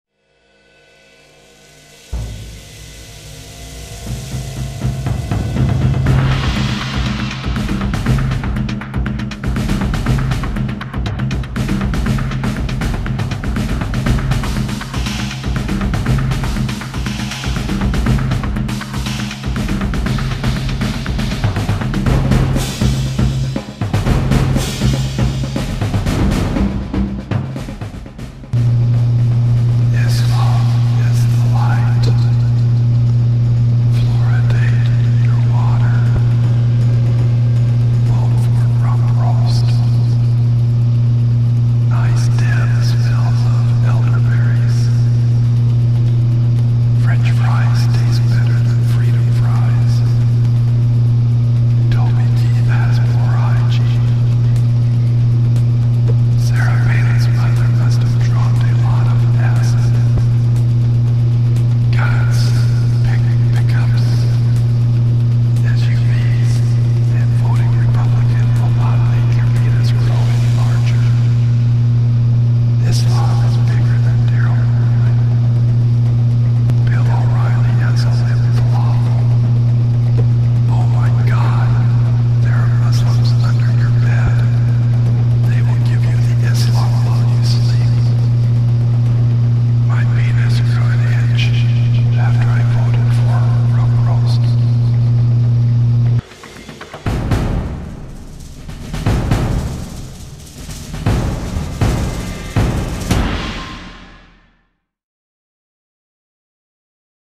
Secret Islamic Messages on the Blogs, Rumproast and Driftglass
Wondering why nicedeb and The Anchoress were losing so badly to Rumproast and Driftglass in the Weblog Awards voting, I checked out the latter's blogs and noticed that my computer's fan sounded
strange. I put a microphone up next to it, and this is what I heard.